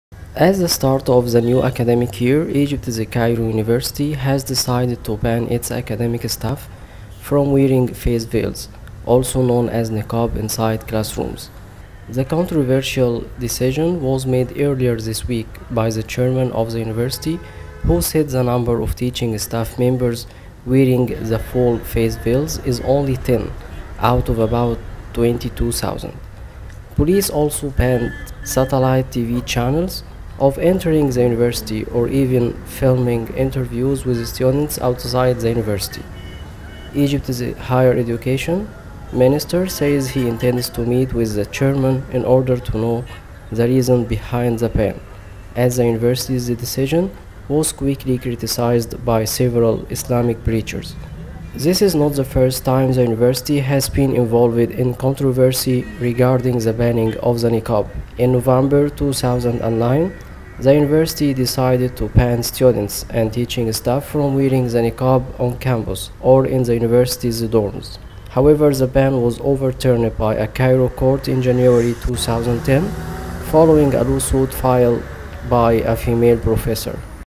TRƯỜNG ĐẠI HỌC CAIRO CẤM GIÁO VIÊN ĐEO MẠNG CHE MẶT
Trường đại học Cairo ở Ai Cập mới đây ra quyết định cấm giáo viên đeo mạng che mặt, hay còn gọi là niqab, trong lớp học. Việc cấm này được giải thích là nhằm giúp việc giao tiếp giữa giáo viên và học sinh được dễ dàng hơn. Lệnh cấm này ngay lập tức nhận được sự chỉ trích từ những người theo đạo Hồi.